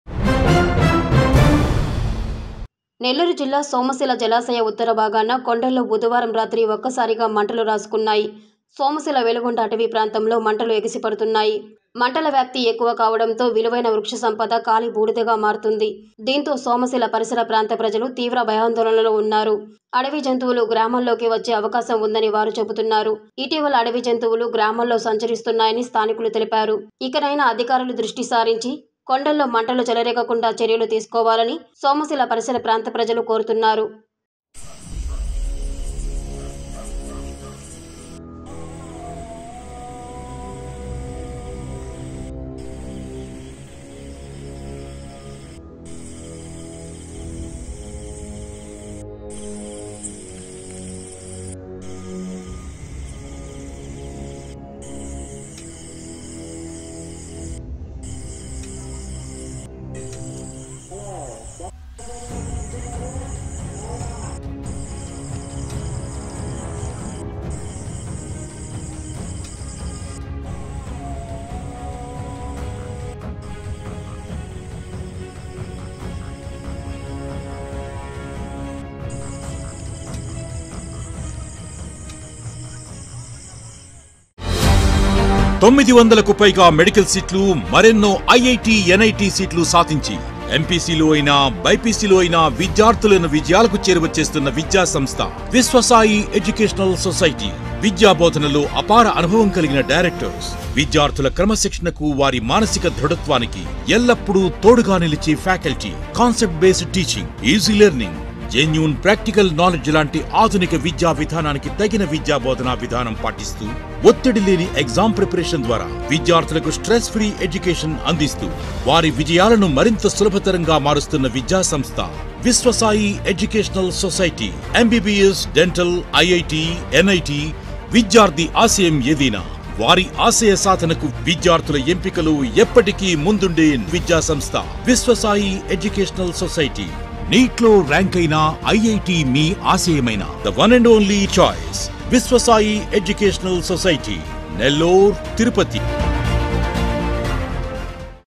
సోమ‌శిల జ‌లాశ‌య పైభాగాన కొండ‌ల్లో కార్చిచ్చు
కాలి బూడిదవుతోన్న విలువైన వృక్ష సంపద
సోమశిల వెలుగోండ అటవి ప్రాంతంలో ఎగిసి పడుతున్న మంటలు
తరచుగా అడవులు తగలపడుతుండటంతో  గ్రామాల్లోకి వస్తున్న అడవి జంతువులు